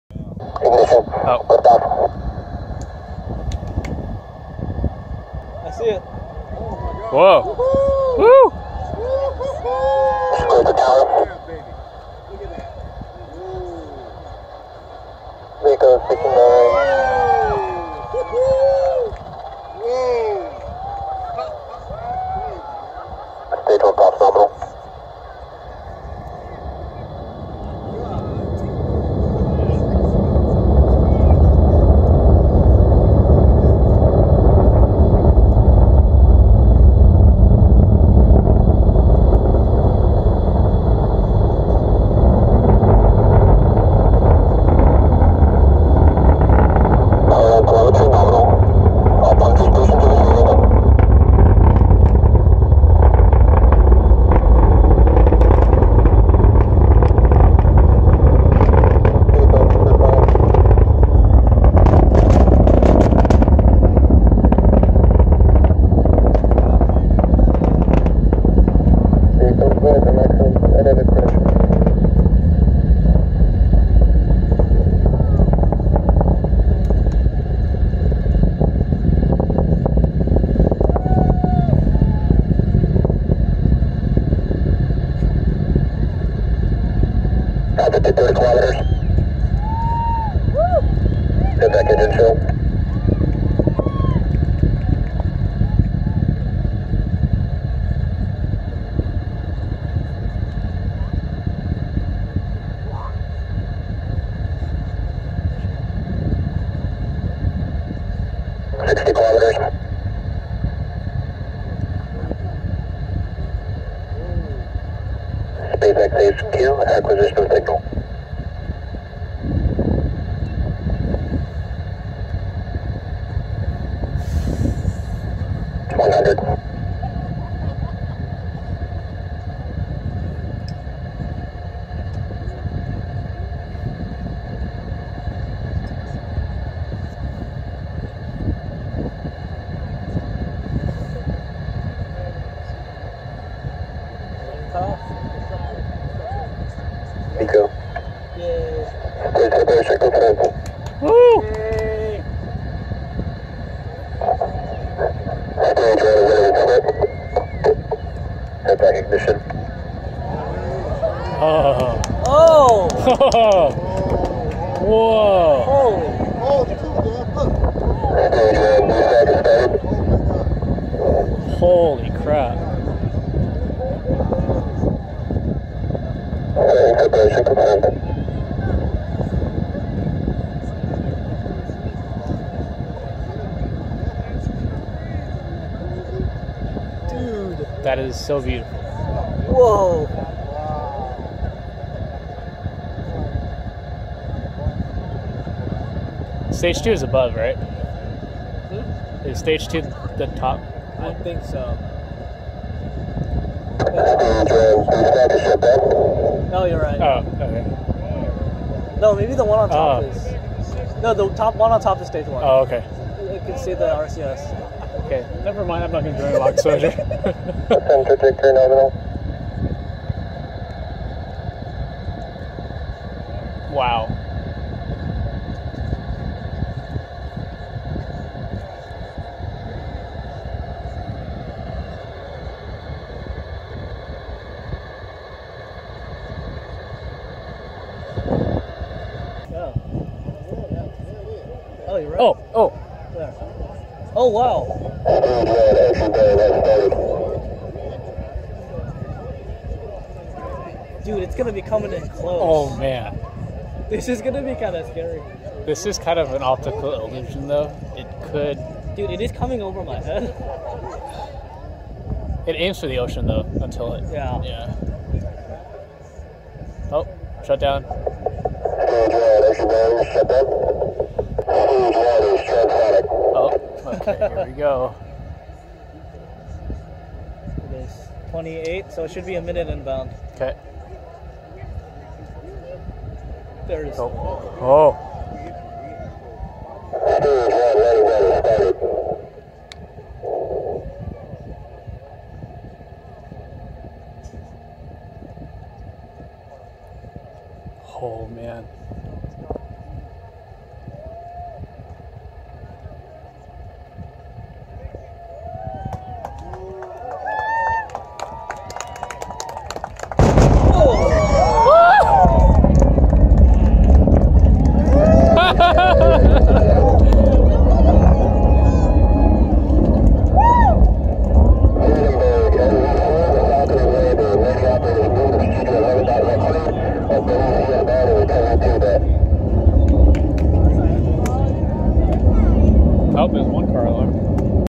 SpaceX Saocom 1A Launch/Landing (Timestamps in Description)
Notable events:
@0:00 Liftoff
@2:38 Illumination of second stage engine plume begins
@4:00 Reentry burn
@4:40 Landing burn
@5:09 Sonic booms